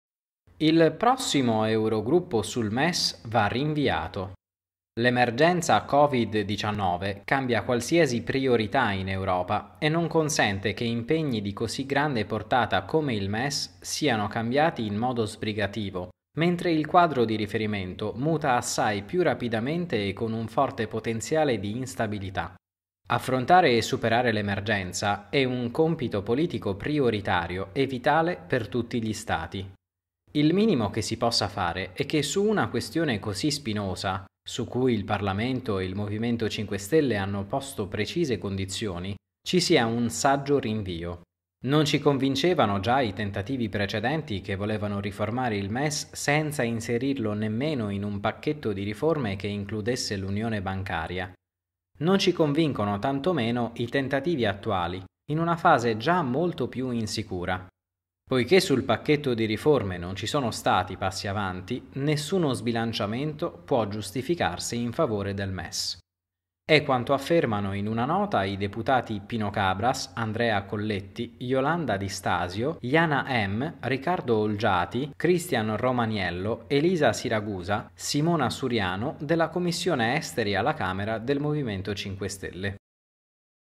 M5S: «L'emergenza coronavirus cambia qualsiasi priorità. Il prossimo Eurogruppo sul MES va rinviato»
«Il prossimo Eurogruppo sul MES va rinviato. L'emergenza COVID19 cambia qualsiasi priorità in Europa e non consente che impegni di così grande portata come il MES siano cambiati in modo sbrigativo mentre il quadro di riferimento muta assai più rapidamente e con un forte potenziale di instabilità. Affrontare e superare l’emergenza è un compito politico prioritario e vitale per tutti gli Stati. Il minimo che si possa fare è che su una questione così spinosa - su cui il parlamento e il MoVimento 5 Stelle hanno posto precise condizioni – ci sia un saggio rinvio. Non ci convincevano già i tentativi precedenti che volevano riformare il MES senza inserirlo nemmeno in un pacchetto di riforme che includesse l’unione bancaria, non ci convincono tanto meno i tentativi attuali in una fase già molto più insicura. Poiché sul pacchetto di riforme non ci sono stati passi avanti, nessuno sbilanciamento può giustificarsi in favore del MES».

È quanto affermano in una nota i deputati Pino Cabras, Andrea Colletti, Iolanda Di Stasio, Yana Ehm, Riccardo Olgiati, Cristian Romaniello, Elisa Siragusa, Simona Suriano, della Commissione Esteri alla Camera del MoVimento 5 Stelle.